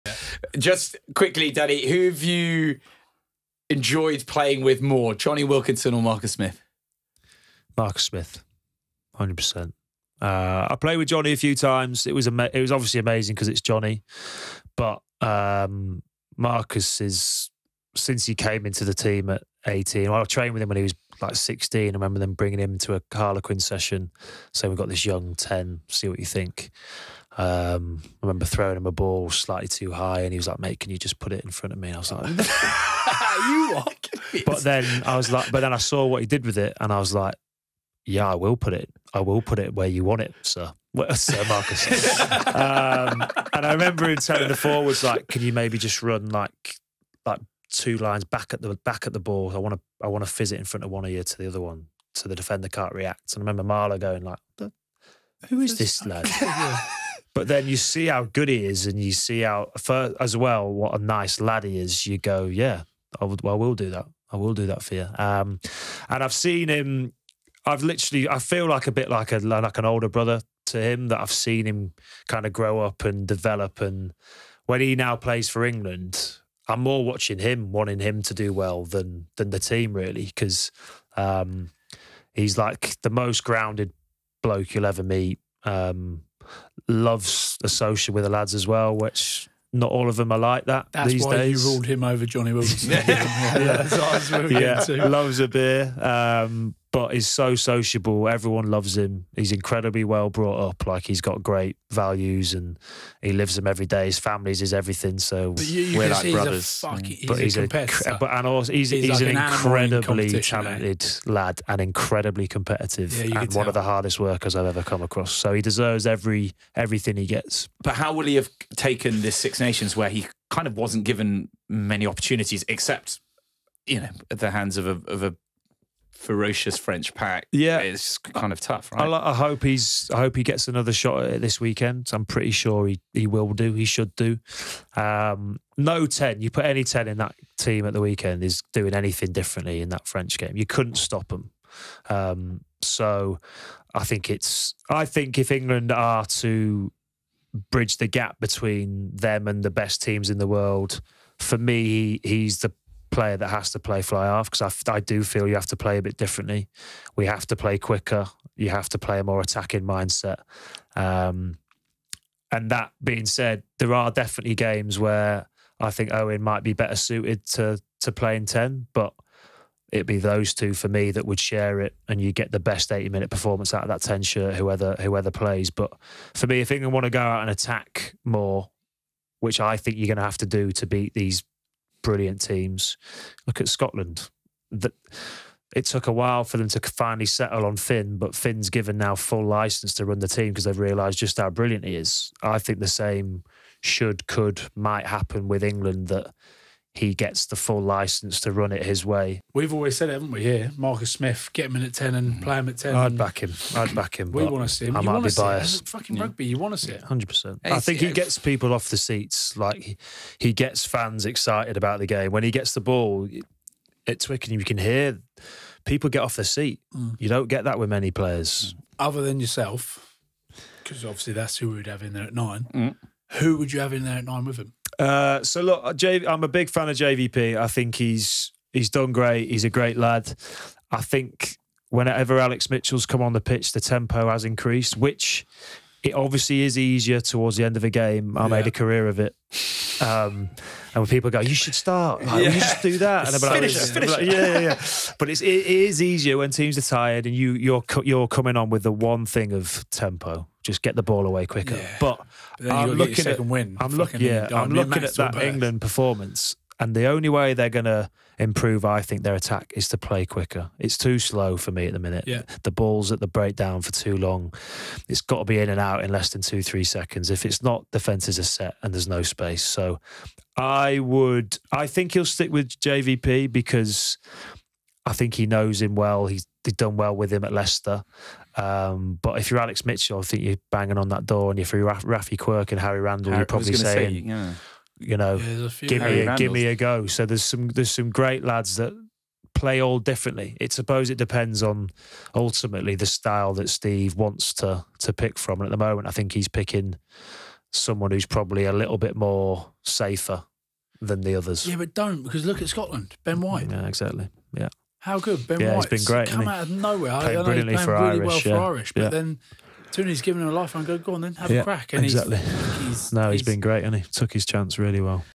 Danny Care gives honest thoughts on teammate Marcus Smith | Offload Podcast
The Offload crew gets into the depths of Marcus Smith and who better to ask than teammate Danny Care. The boys speak on his position and how he's best played for England going forward.